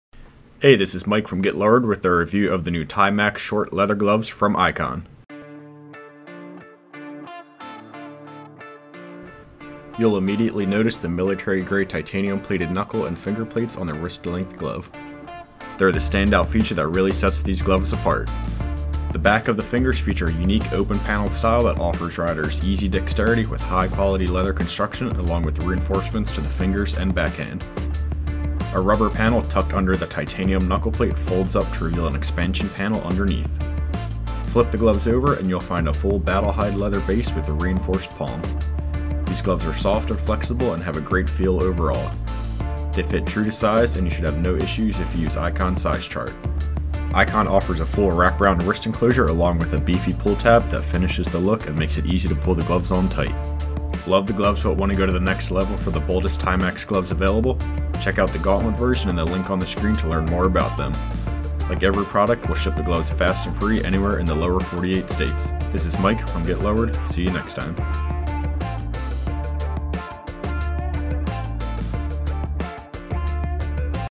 Icon Timax Short Gloves Overview - GetLowered.com
Any questions about these gloves or anything else - just let us know. And remember at Get Lowered Cycles, we ship everything fast and free.